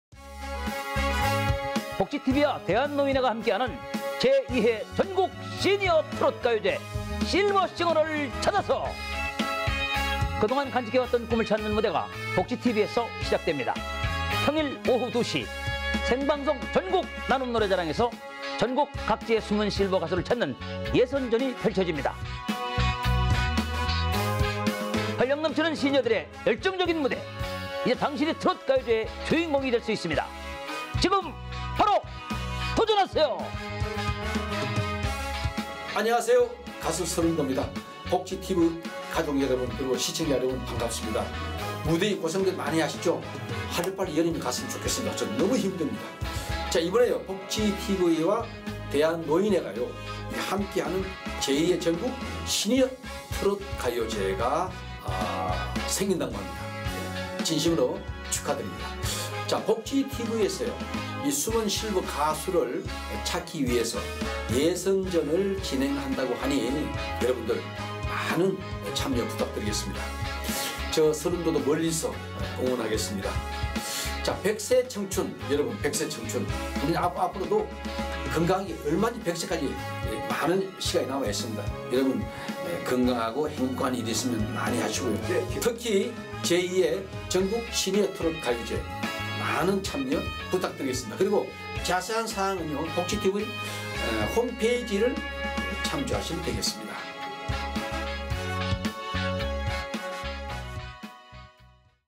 백세청춘!! 액티브 시니어를 위한 '제2회 전국시니어트롯가요제' 예선 참가자를 모집합니다
복지TV에서 제2회 전국 시니어 트롯 가요제 예선 참가자를 모집합니다.

참가자격 – 대한민국 65세 이상 누구나 참여 가능
신청기간 – 7월 22일 ~ 8월2일 오후 5시까지